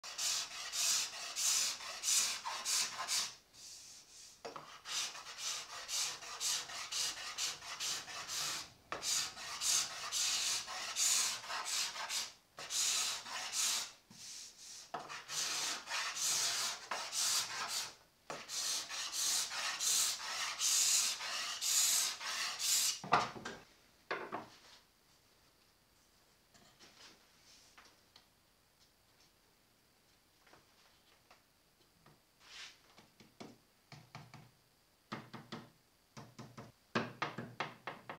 Scraper flattening
Using a card scraper to finish the flat side of a violin plate.
1mm thick, double cutting edge, 90° sharpening angle

Davide Sora® is a registered trademark
Davide Sora® è un marchio registrato